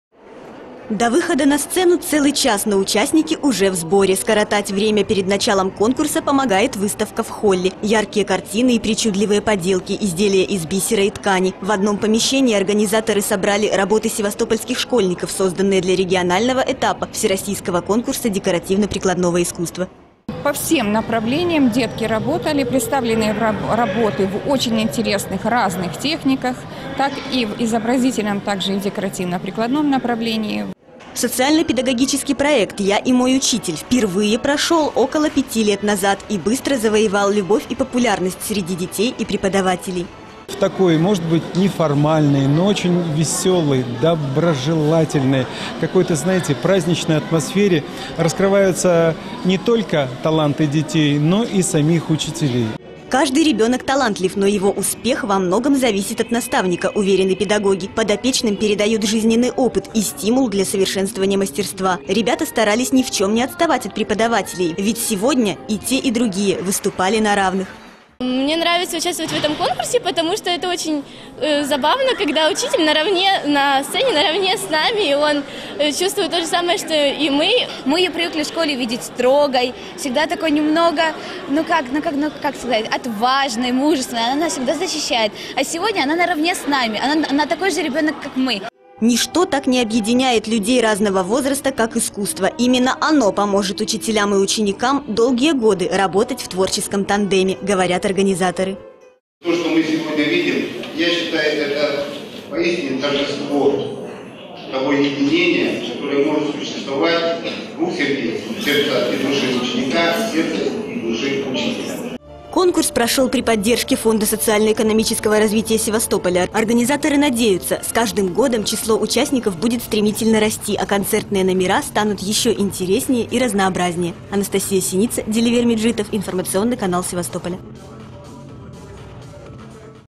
В Севастополе провели финал конкурса «Я и мой учитель»
Во Дворце детского и юношеского творчества провели финальный этап городского конкурса «Я и мой учитель». В творческом тандеме объединились представители  севастопольских школ.  Победителей и призеров наградили грамотами и призами.